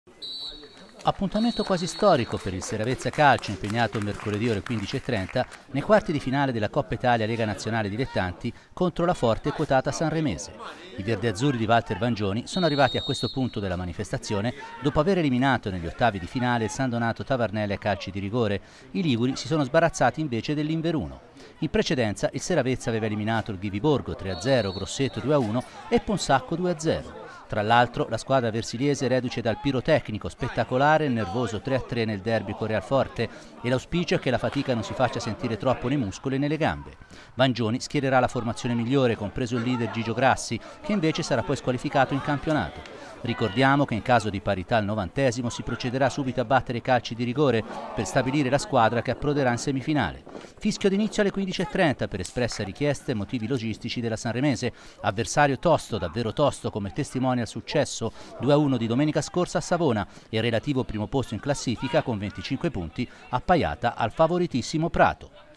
Seravezza ospita la Sanremese nei quarti di finale di Coppa Italia LND
CALCIO D - Appuntamento quasi storico per il Seravezza Calcio impegnato mercoledì (ore 15,30) nei quarti di finale della Coppa Italia LND contro la quotata Sanremese. I verdeazzurri di Walter Vangioni sono arrivati a questo punto della manifestazione dopo aver eliminato negli ottavi il San Donato Tavarnelle ai calci di rigore. Iliguri si sono sbarazzati dell'Inveruno.

NoiTv